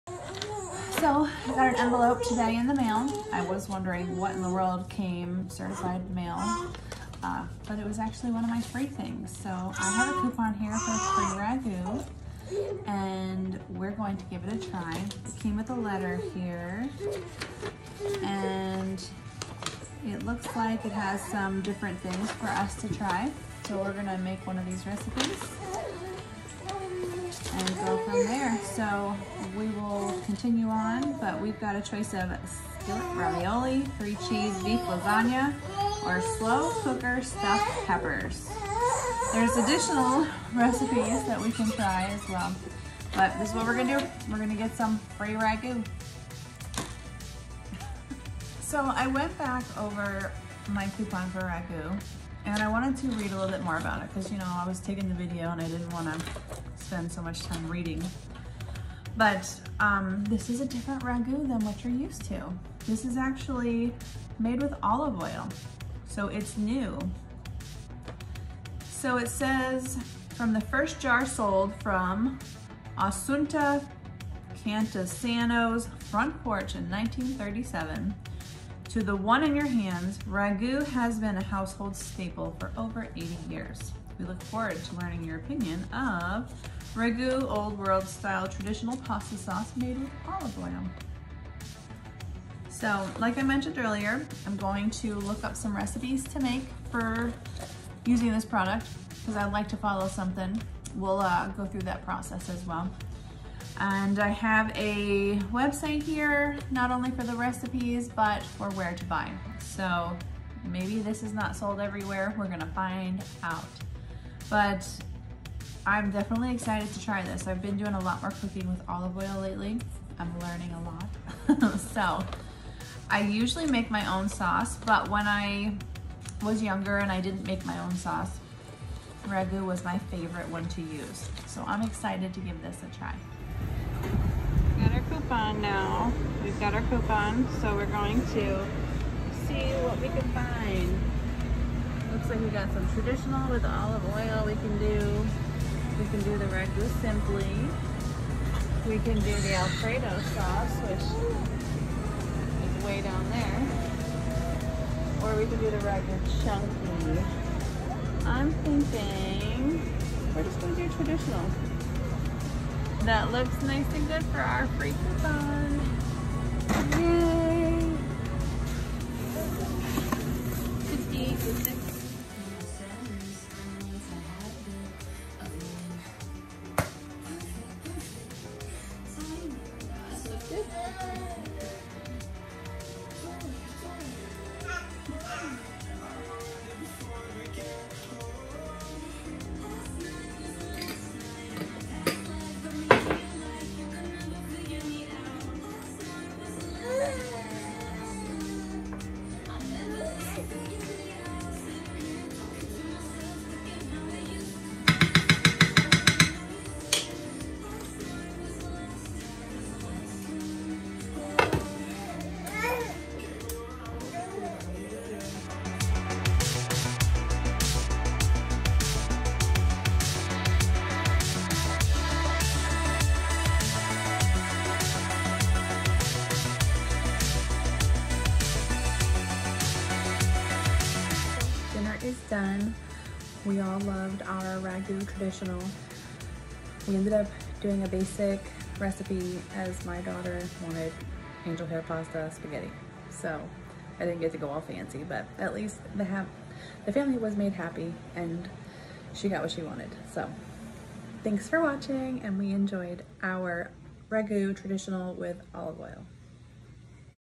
Ragu Old World Style Traditional Sauce
Ragu Old World Style Traditional Sauce sampling with my family. It now is made with Olive Oil and it was very good. We were excited to try this one and we were glad to eat the left overs.